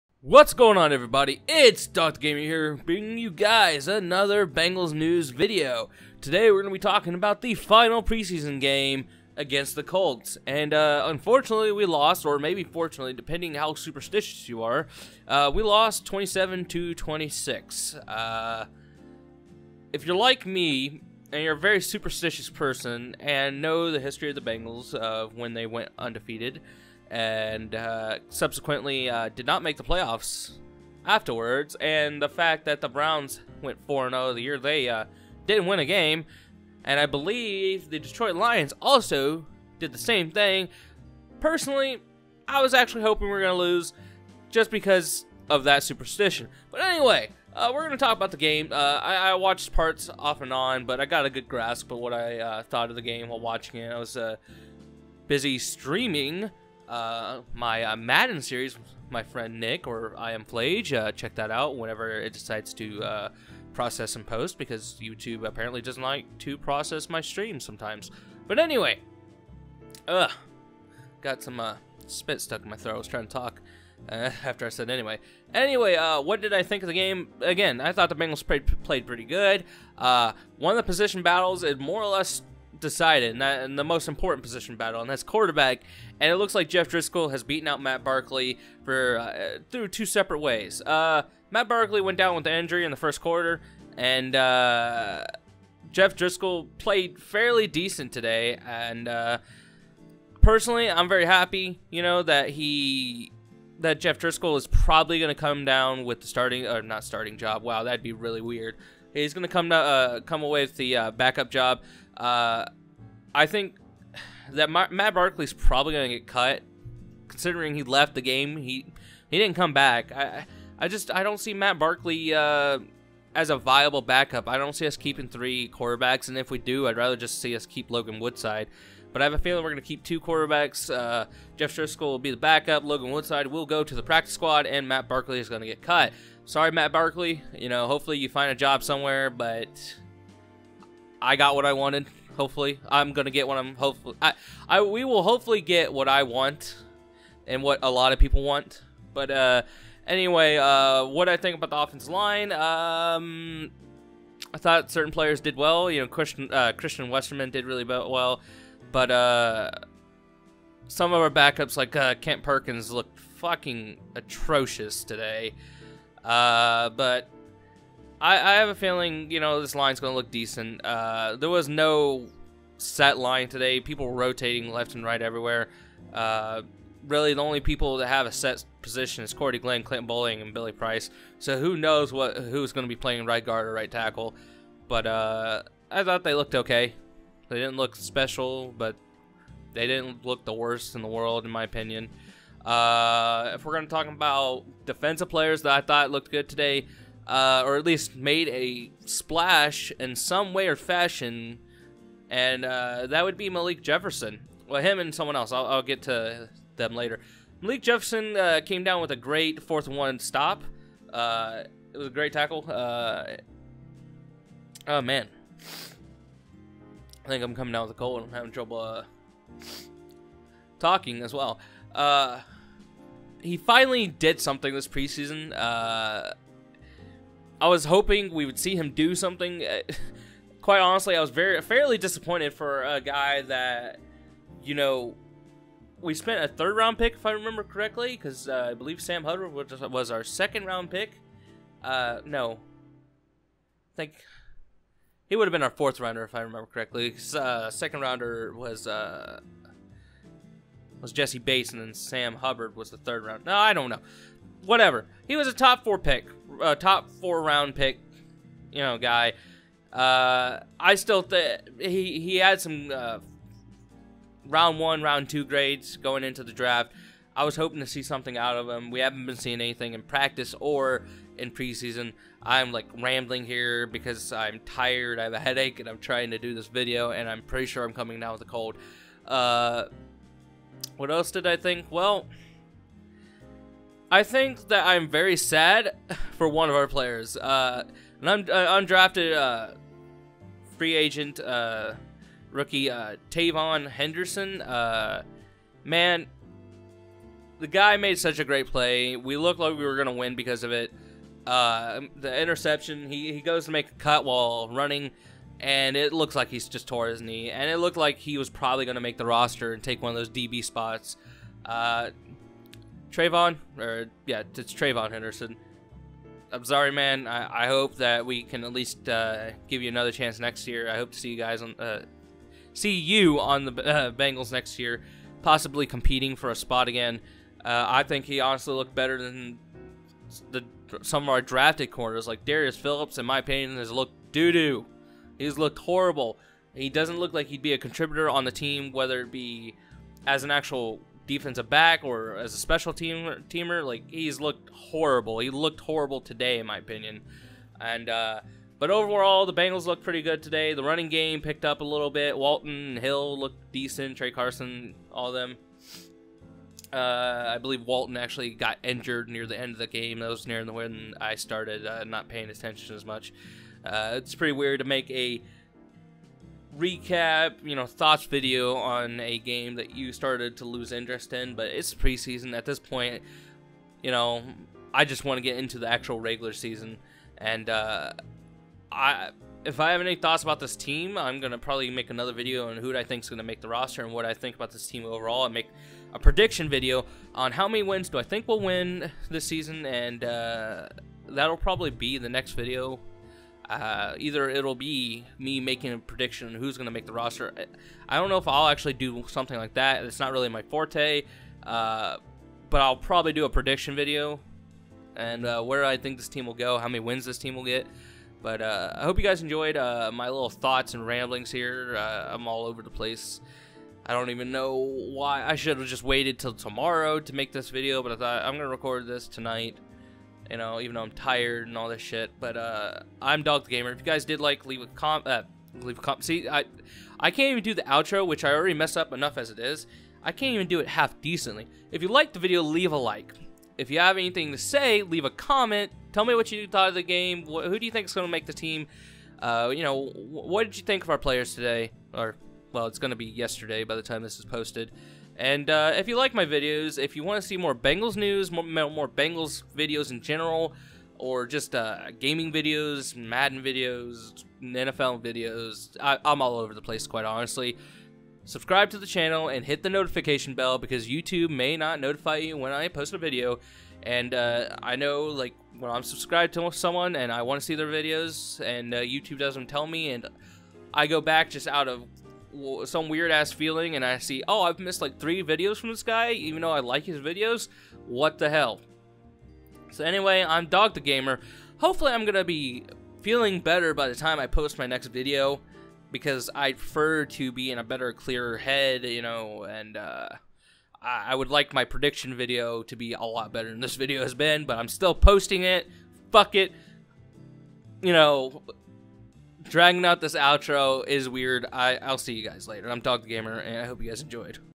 Bengals VS Colts Reaction/Recap | 2018 NFL Preseason Week 4 | Bengals News
Today we will be talking about the Cincinnati Bengals VS Indianapolis Colts in their Preseason Week 4 Match up. Bengals VS Colts 27-26 Loss.